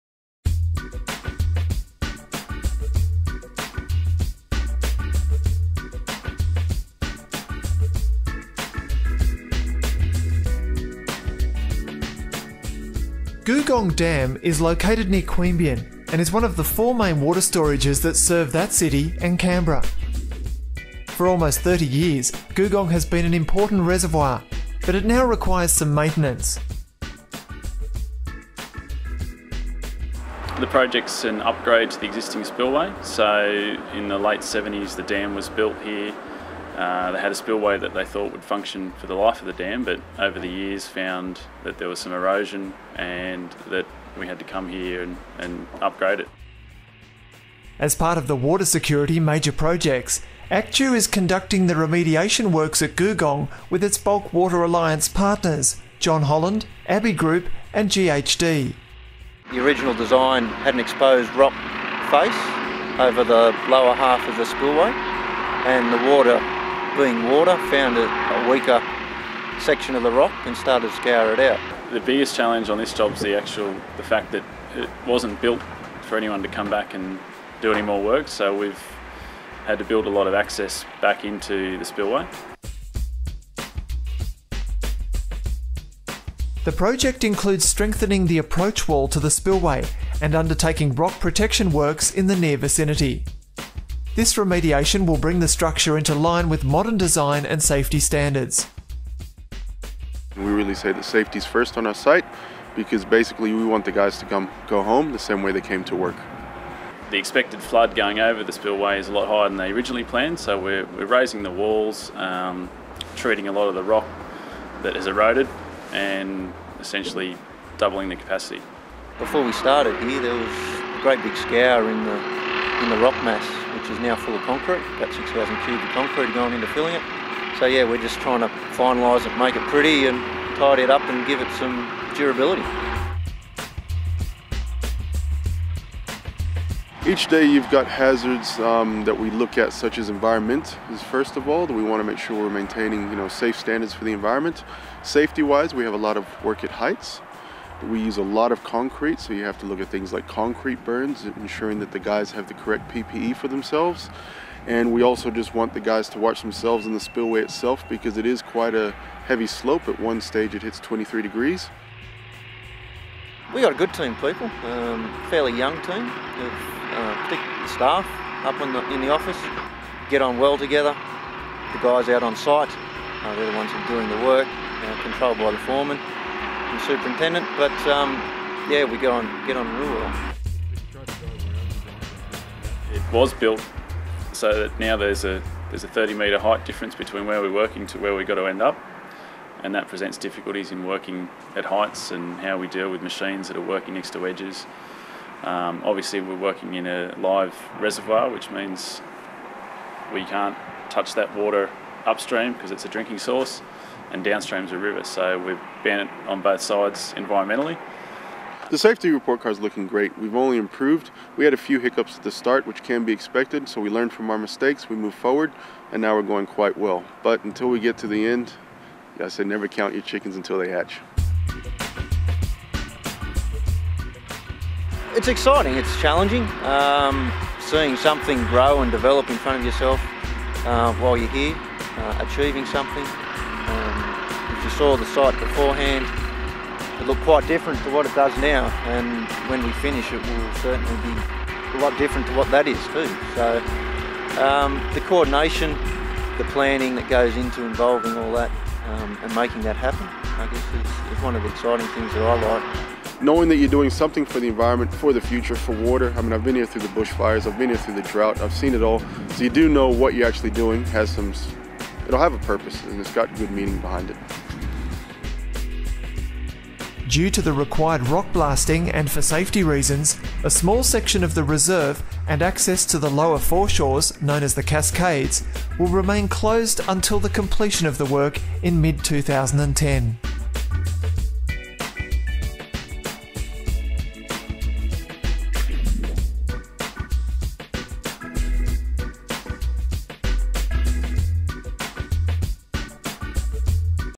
Googong Dam Spillway works - September 2009
ACTEW's water security projects include other important works that sometimes get overshadowed by the scale of the Enlarged Cotter Dam. One of these is the Googong Dam Spillway, a major remediation project at the dam which is located near Queanbeyan. Along with the other ACT dams, Googong is a very important water storage reservoir for both Canberra and Queanbeyan.

[Documentary maker Richard Snashall works with ACTEW Corporation to help us capture and document the Enlarged Cotter Dam and other water security projects for our Heritage Archive.]